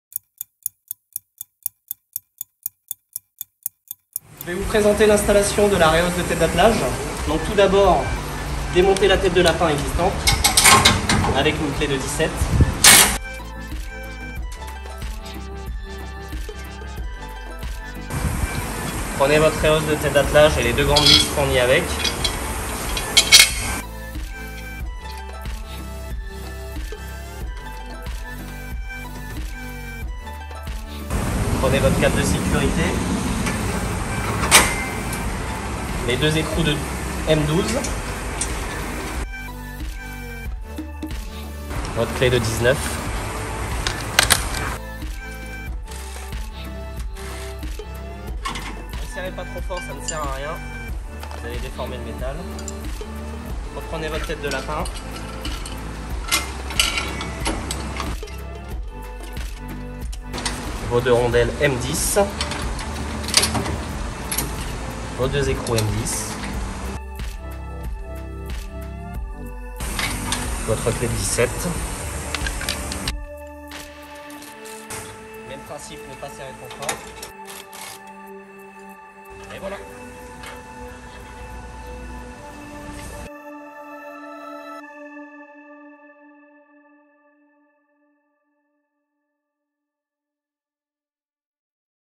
MONTAGE REHAUSSE DE TÊTE D'ATTELAGE (By Remorque Import)
Découvrez nôtre vidéo explicative,
ici montage de la rehausse de tête d'attelage .
Bonne utilisation !